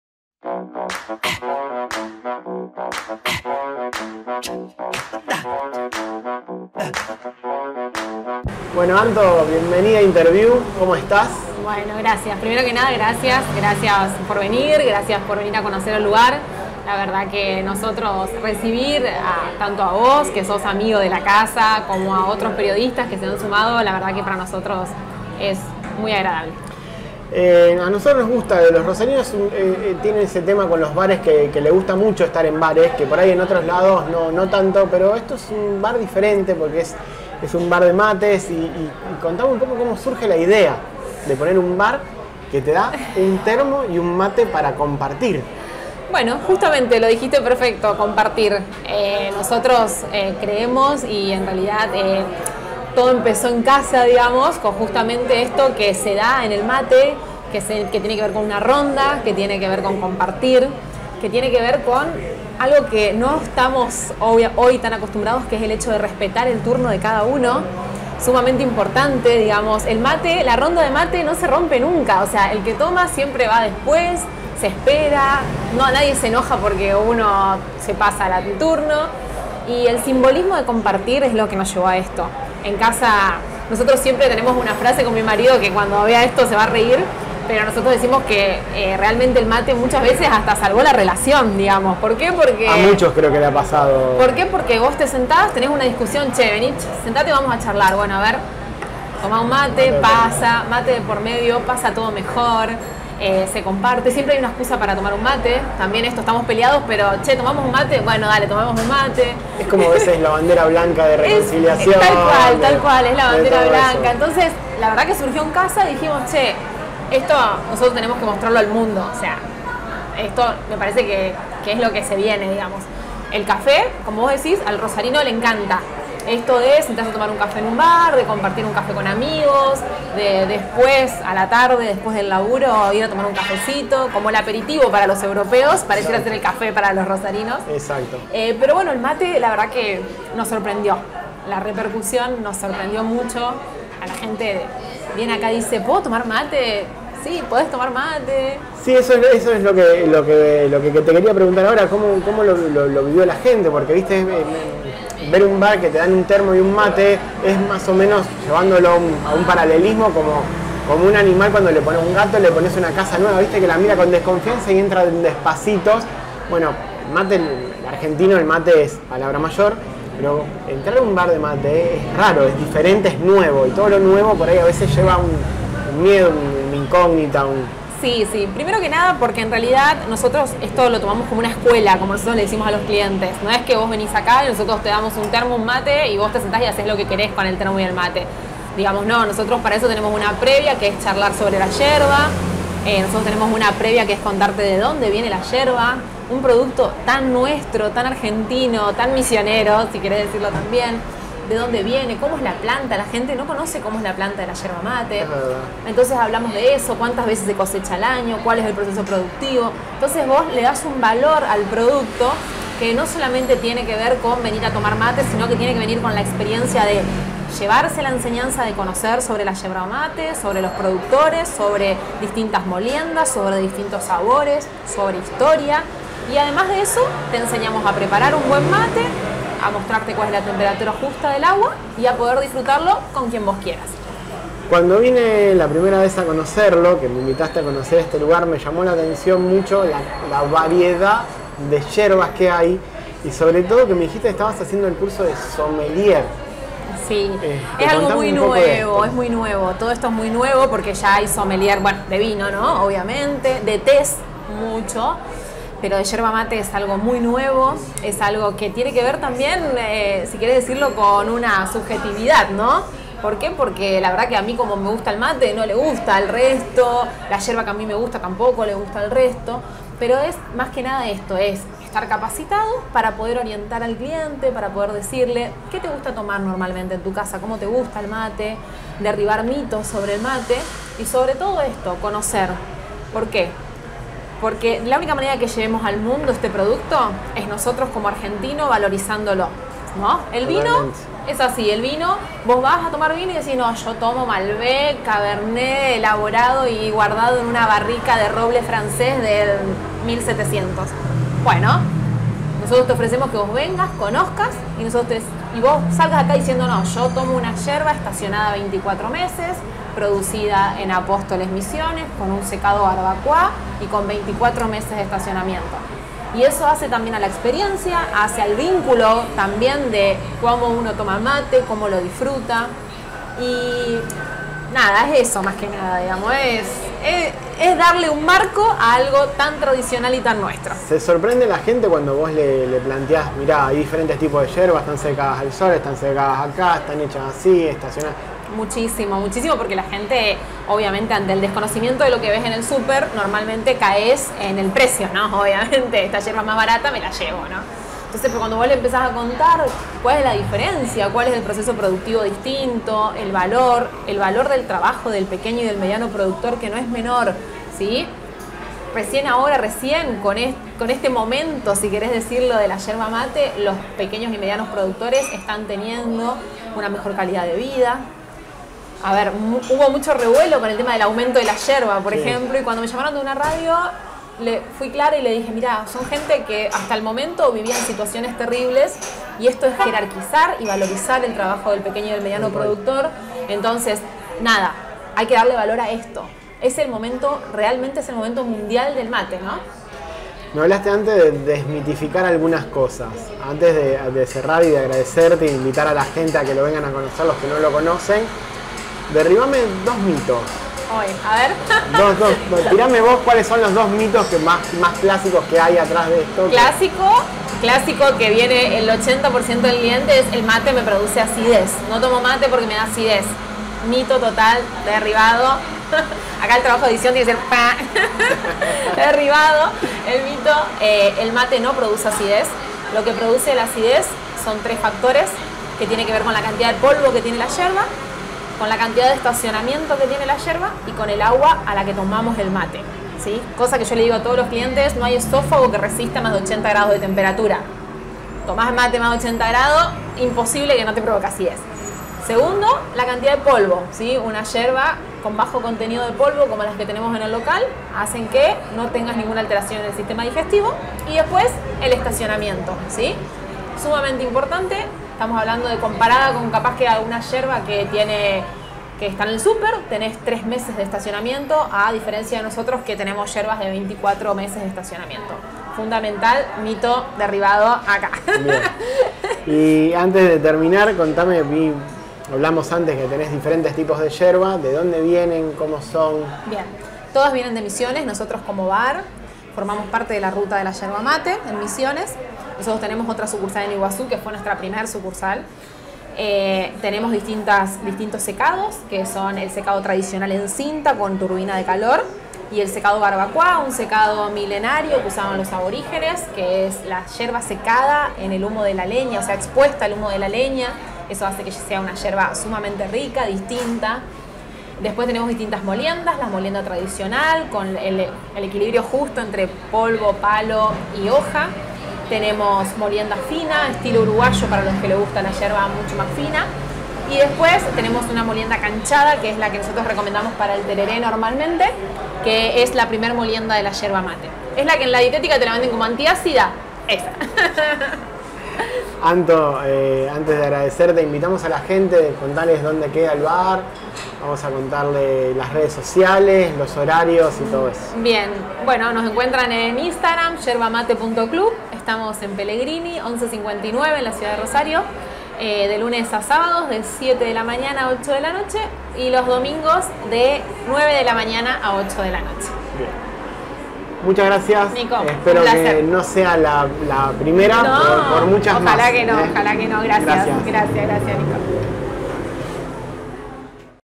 Interviu en Yerba Mate Club
Nico Llanos charló con Antonella dueña de este particular bar en la ciudad de Rosario